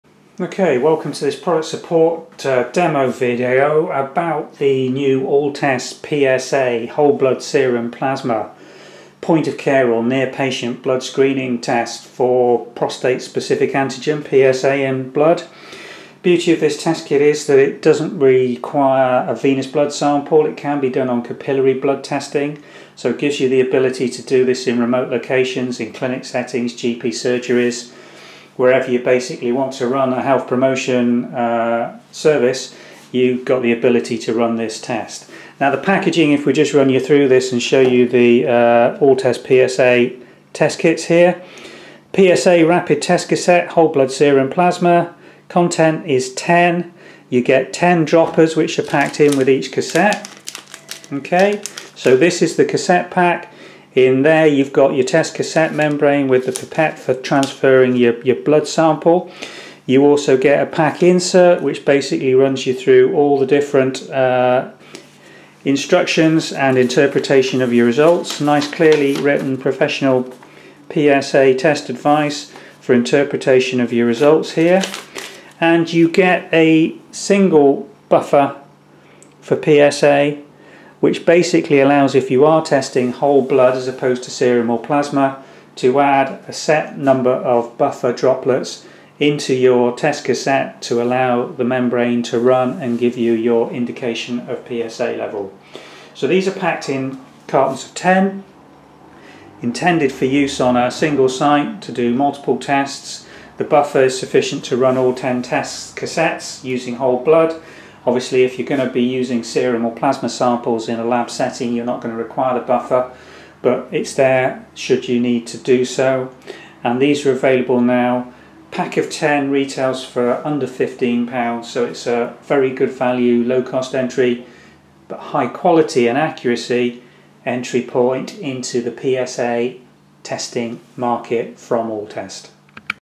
How to test Prostate Specific Antigen PSA
Point of care testing for PSA is fast reliable and easy with new ALLTEST point of care rapid test cassettes for PSA in packs of 10 tests